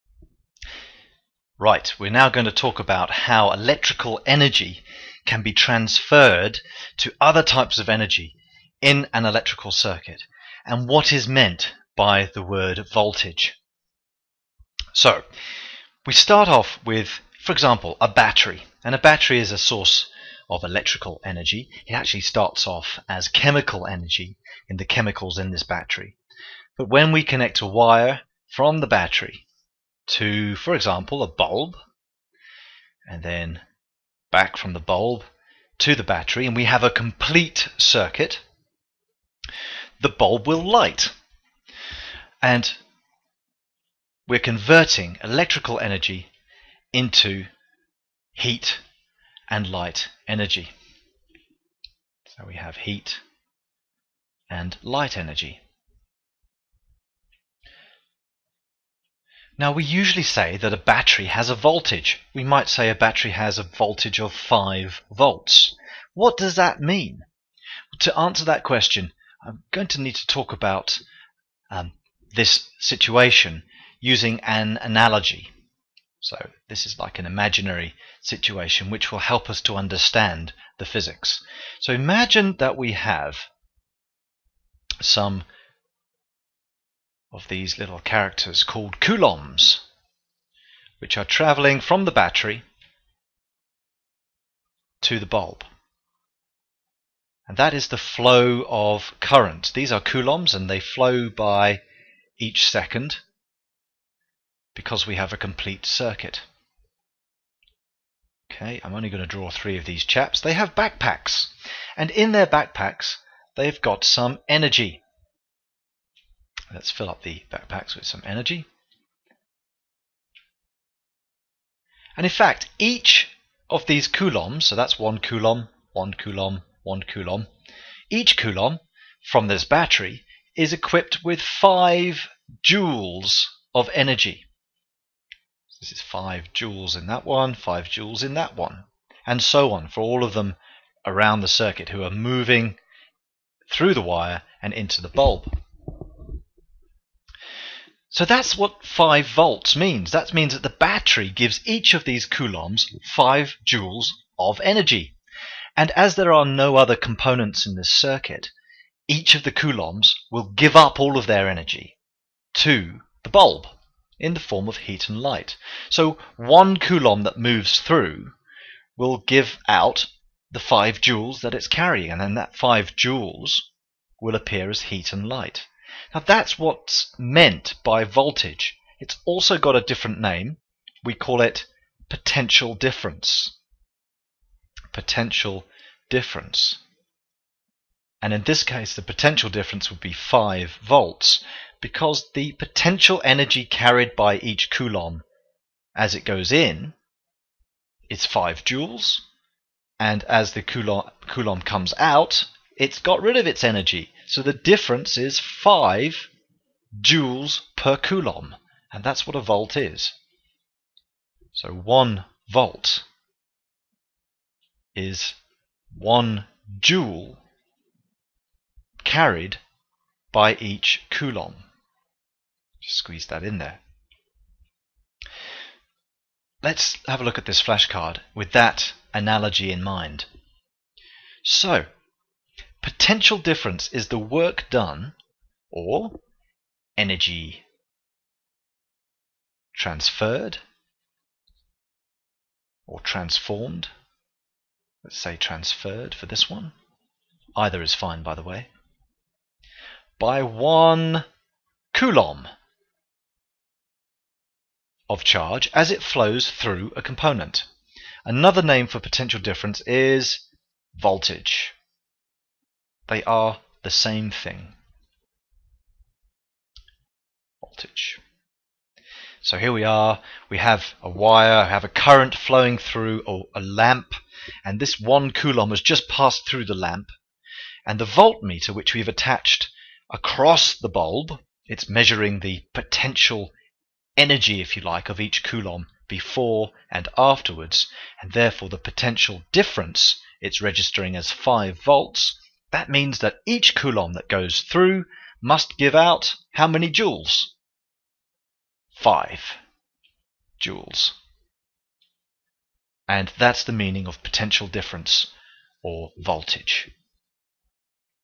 The Meaning of Potential Difference - Electrical Circuits - GCSE Physics
This physics tutorial explains how electrical energy carried by a coulomb of charge can be transferred from a cell or battery to other components in a circuit.